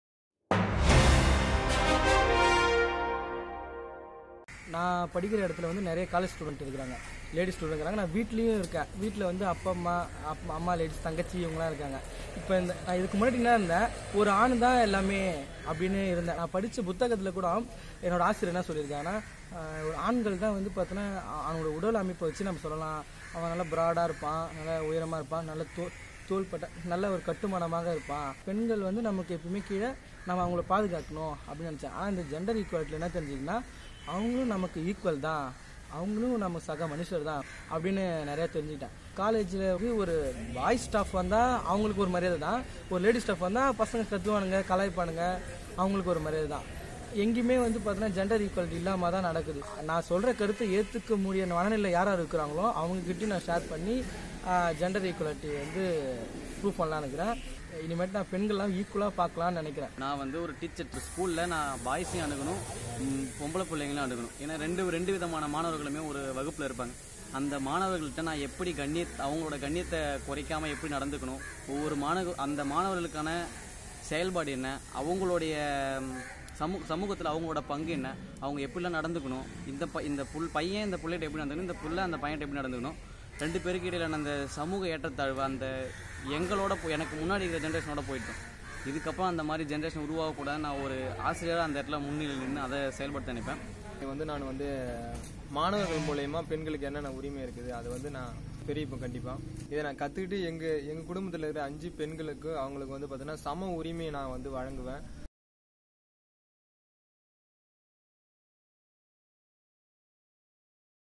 Gender Mainstreaming – Youth Perspectives
Indian Social Institute, Bengaluru (ISI-B) organized trainings on "Gender Mainstreaming" in 7 districts of Tamil Nadu for rural youth to usher in attitudinal changes among the participants and spread the knowledge among the communities.